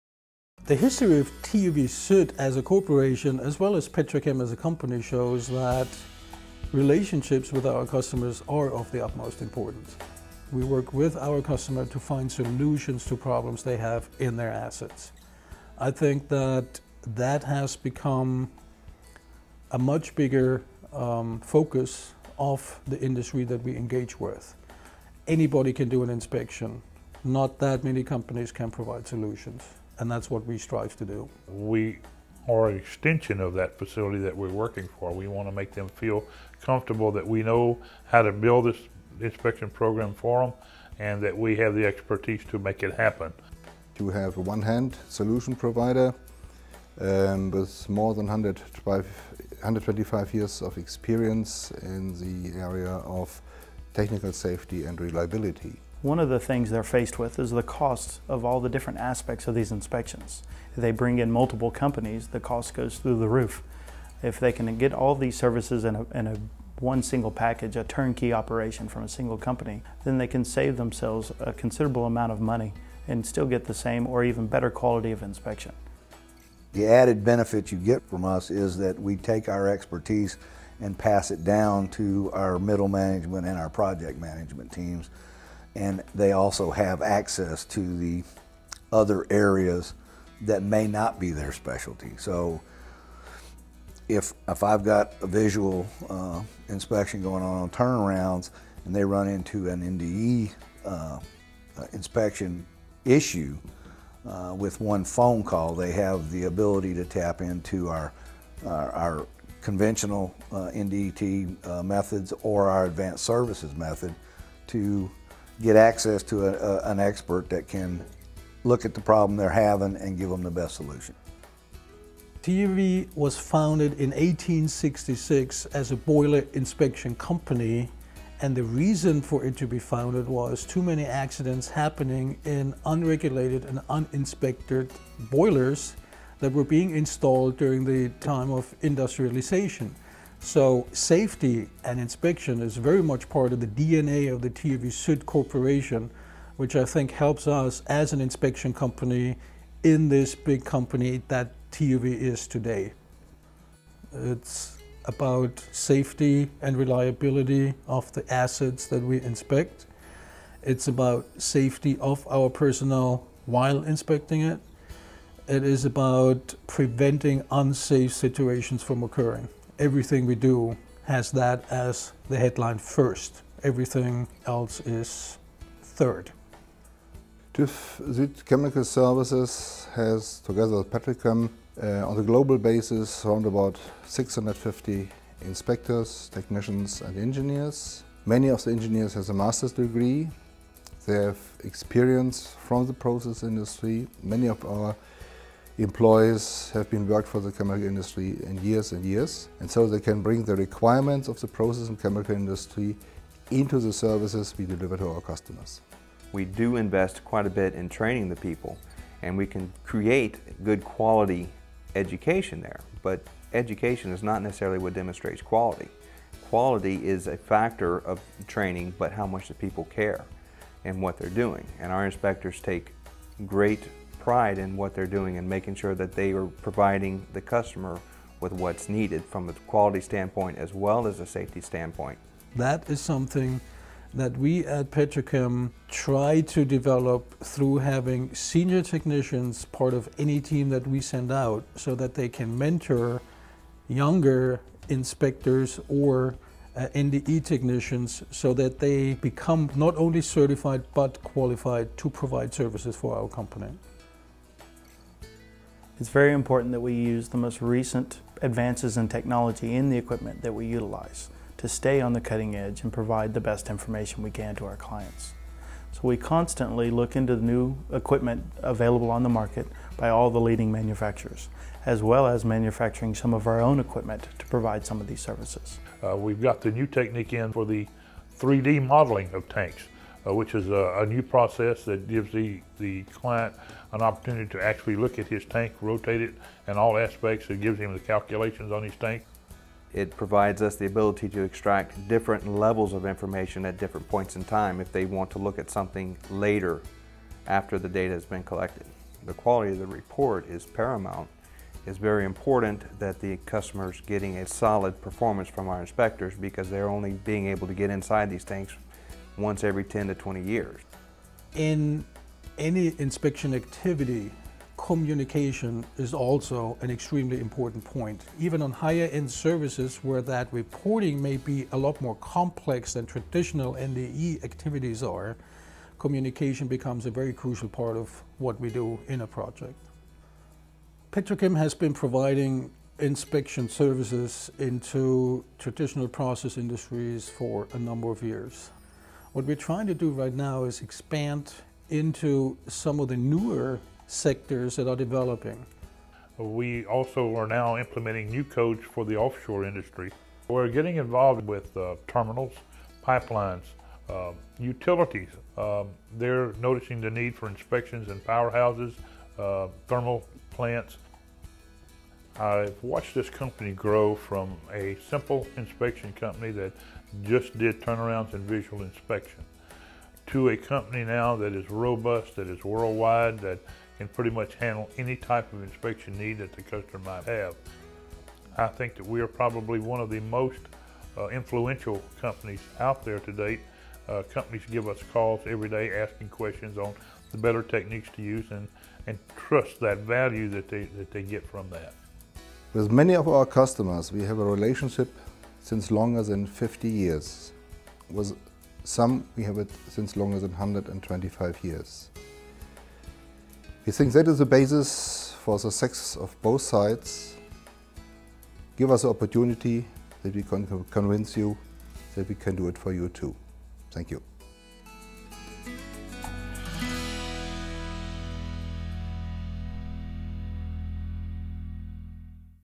PetroChem Company Overview
PetroChem Inspection Services is a worldwide leader in providing outsourced turnaround and maintenance inspection personnel and services to the petrochemical industry.
PetroChem specializes in advanced Non-destructive Testing (NDT) services such as Tube Testing (ECT, RFT, IRIS), Real Time Radiography (RTR), Computerized Radiography, Phased Array Ultrasonics, and Alternating Current Field Measurement (ACFM).